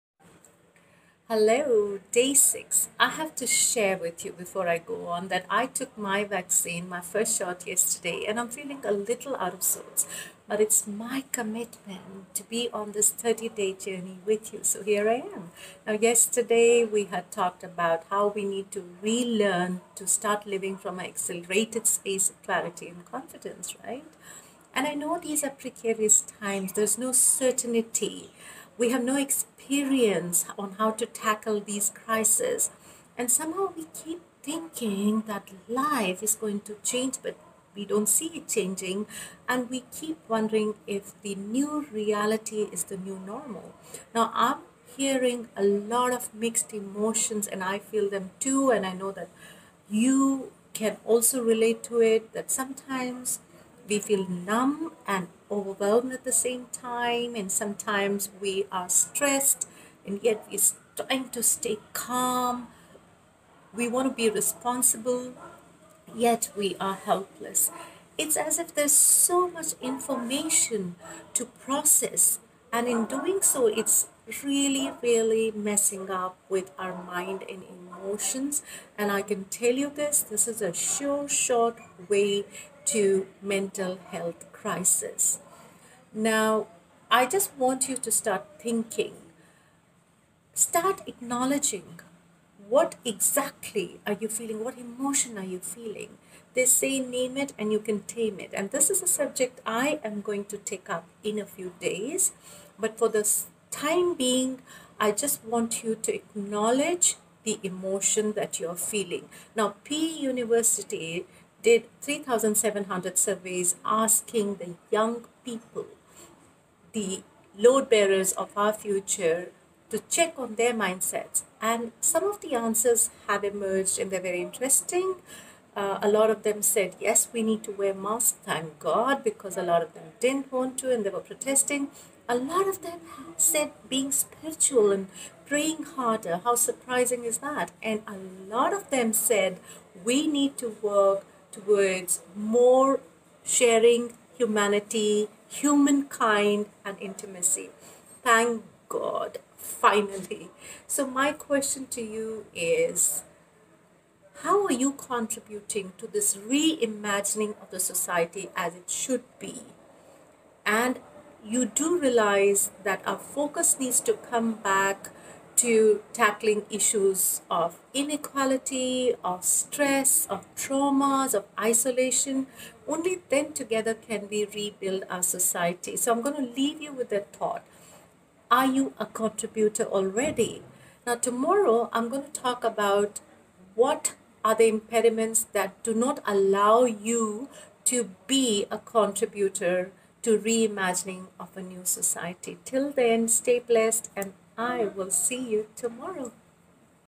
Day 6 – Re-imagining society to move to joy.
We are feeling mixed emotions. A mix of completely numb and absolutely overwhelmed all at the same time.
Responsible and helpless. Invigorated YET exhausted. Stressed and at times calm.
It’s like there’s way too much to process, but absolutely nothing happening in each moment.
Doesn’t it sound as if it’s time to Re-imagine our society?